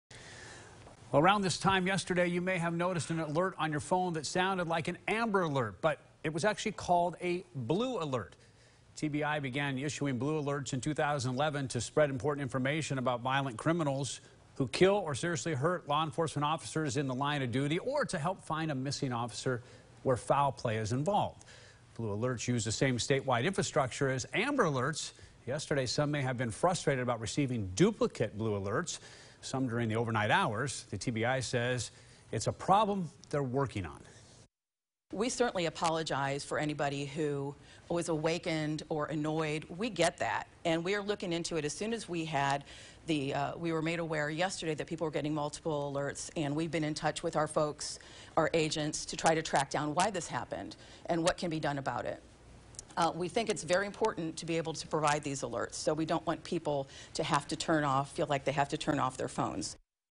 What is a Blue Alert?
Among the various alerts from law enforcement that you may notice flashing across your cell phone screen or appearing in your social media feeds is the Blue Alert.